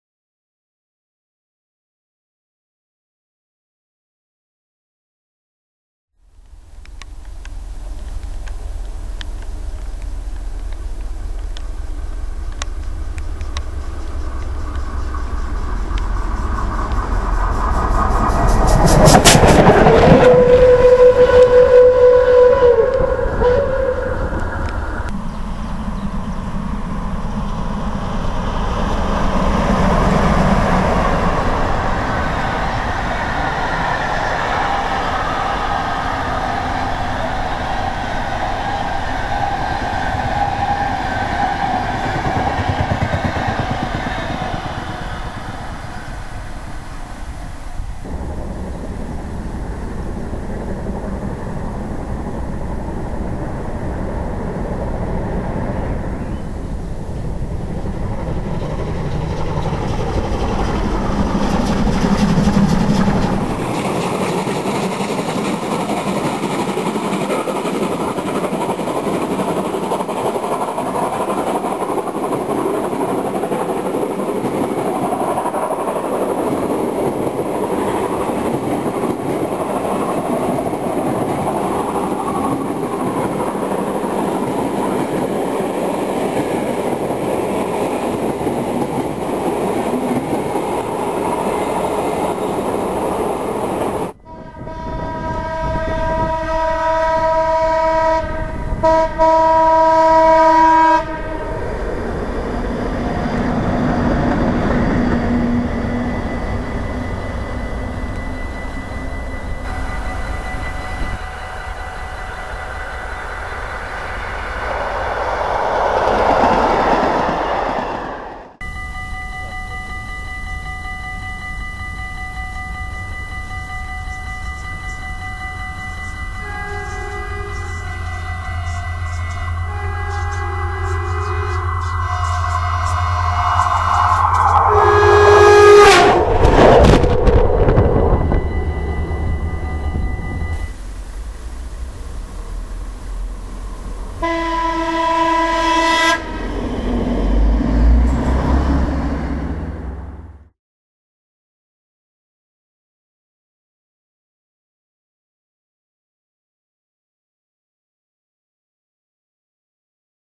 Van oude treinen die voorbijgaan
Oude treinen door Bunnik, Houten en Utrecht: loc 065 018, loc 01 1066, Blokkendoos, loc 1202, Blauwe Engel 41